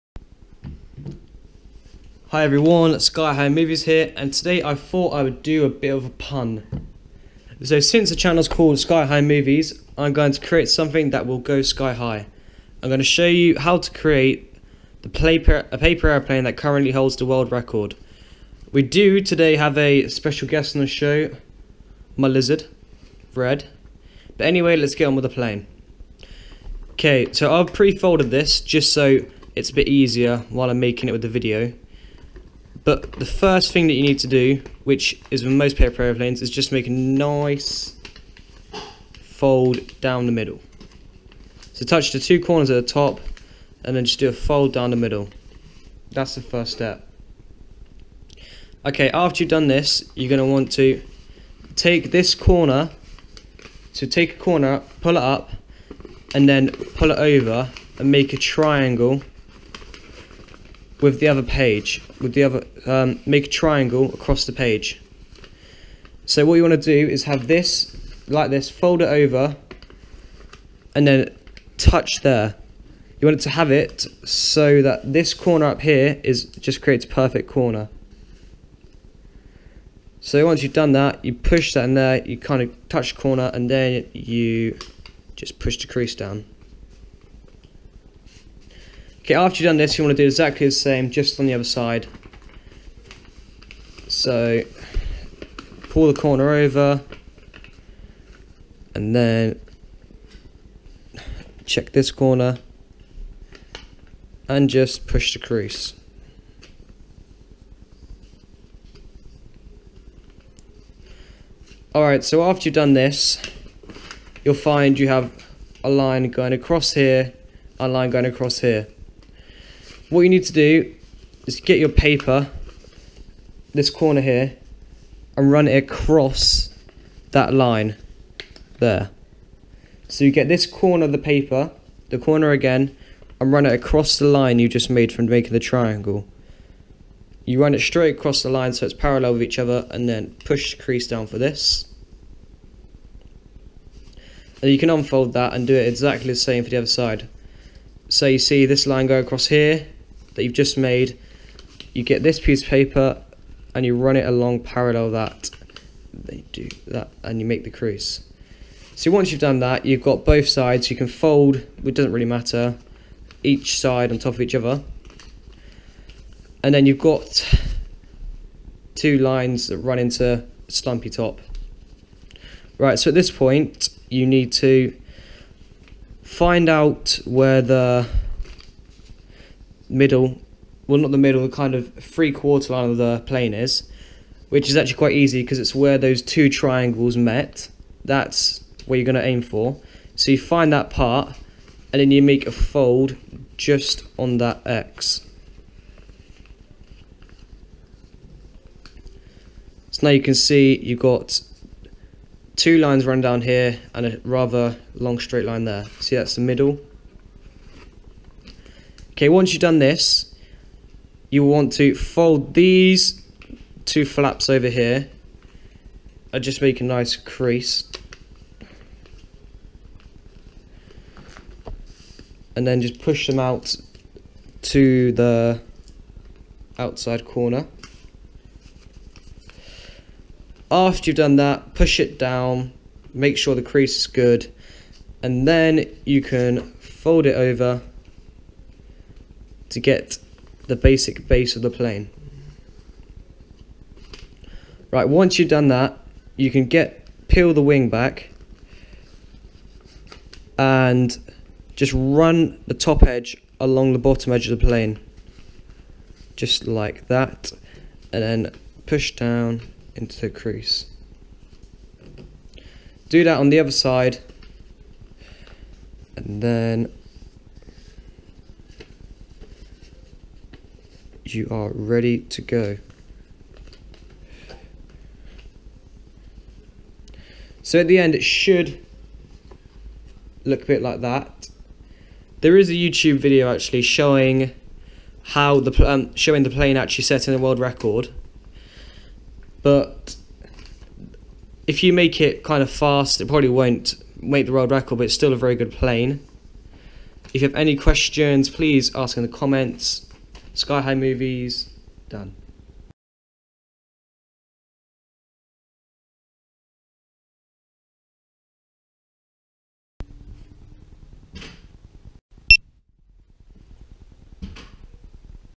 best paper airplane in the world
i did not create this airplane
i am just showing you how to make it. if you would like to see the plain breaking the record pleas search paper plane makes history